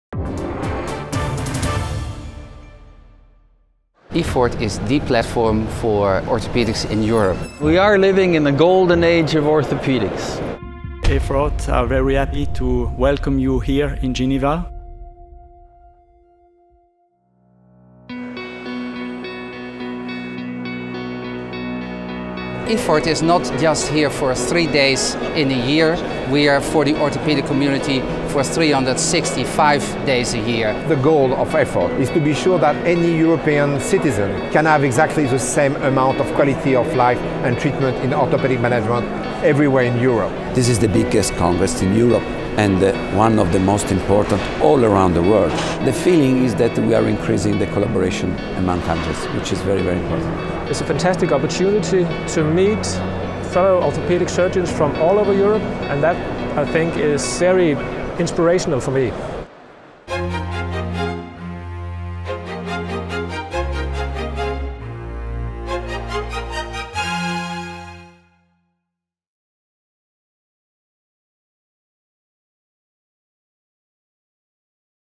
EFORT in the Future
Driving Orthopaedics & Traumatology management: how sharing knowledge will benefit citizens in the same access to Orthopaedic & Traumatology treatment.